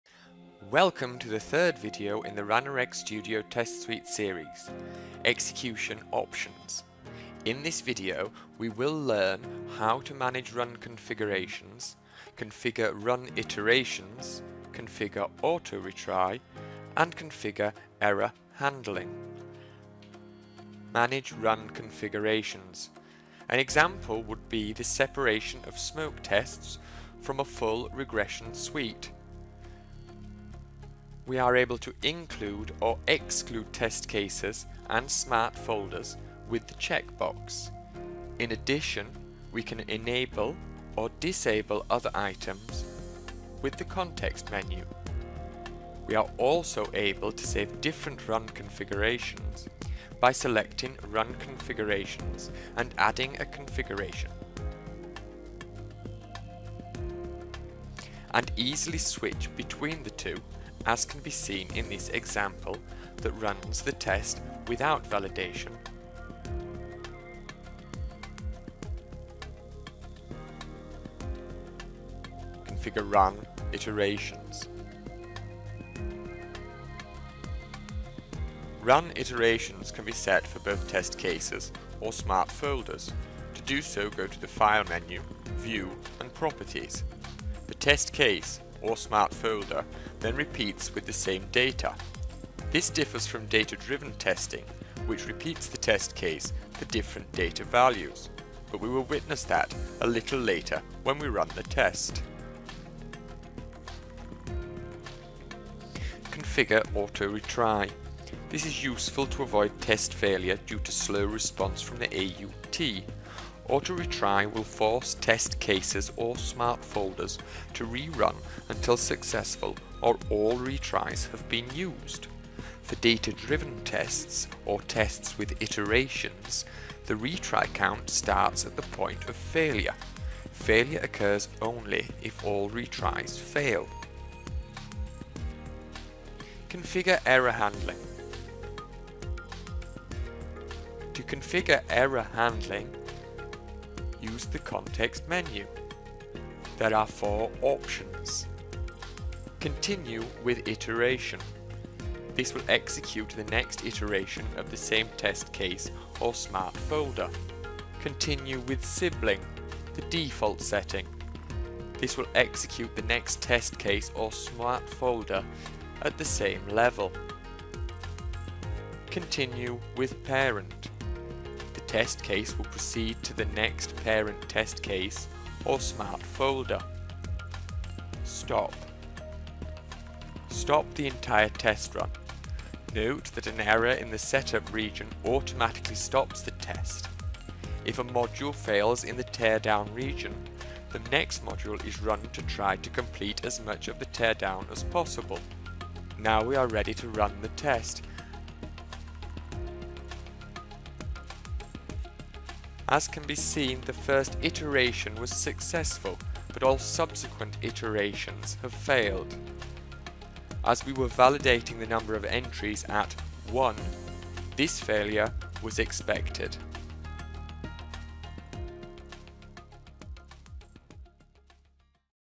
Ranorex Studio Test Suite 3/4: Test suite execution options
This video will talk you through the options available when executing a test. The accompanying chapter in the user guide can be found here: